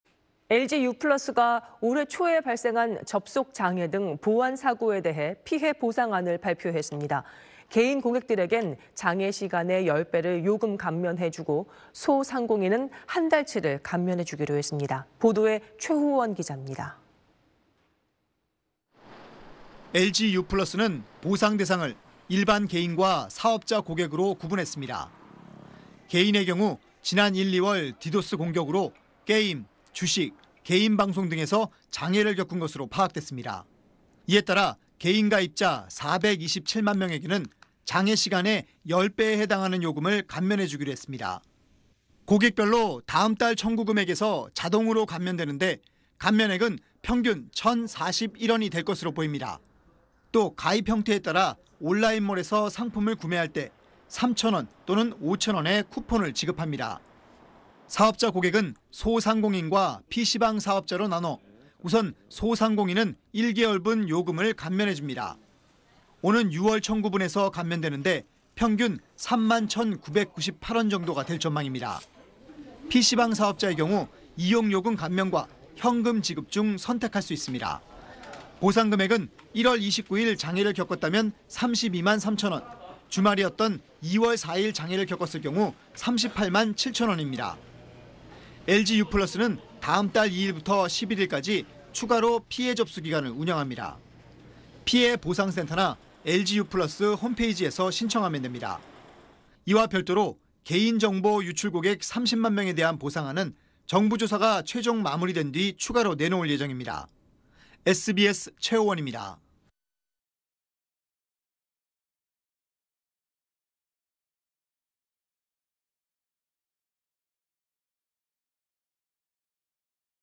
LG유플러스, 접속 장애 보상안 발표…10배 · 1개월 감면 / SBS
〈앵커〉

LG유플러스가 올 초에 발생한 접속 장애 등 보안 사고에 대해 피해보상안을 발표했습니다. 개인 고객들에겐 장애 시간의 10배를 감면해 주고, 소상공인은 한 달 치를 감면해 주기로 했습니다.

보도에 최호원 기자입니다.

〈기자〉

LG유플러스는 보상 대상을 일반 개인과 사업자 고객으로 구분했습니다.

개인의 경우 지난 1, 2월 디도스 공격으로 게임, 주식, 개인방송 등에서 장애를 겪은 것으로 파악됐습니다.

이에 따라 개인 가입자 427만 명에게는 장애 시간의 10배에 해당하는 요금을 감면해 주기로 했습니다.

고객별로 다음 달 청구 금액에서 자동으로 감면되는데, 감면액은 평균 1천41원이 될 것으로 보입니다.

사업자 고객은 소상공인과 PC방 사업자로 나눠 우선 소상공인은 1개월분 요금을 감면해 줍니다.

오는 6월 청구 분에서 감면되는데, 평균 3만 1천998원 정도가 될 전망입니다.

PC방 사업자의 경우 이용 요금 감면과 현금 지급 중 선택할 수 있습니다.

보상 금액은 1월 29일 장애를 겪었다면 32만 3천 원, 주말이었던 2월 4일 장애를 겪었을 경우는 38만 7천 원입니다.

LG유플러스는 다음 달 2일부터 11일까지 추가로 피해 접수 기간을 운영합니다.

피해보상센터나 LG유플러스 홈페이지에서 신청하면 됩니다.

이와 별도로 개인정보 유출 고객 30만 명에 대한 보상안은 정부 조사가 최종 마무리된 뒤 추가로 내놓을 예정입니다.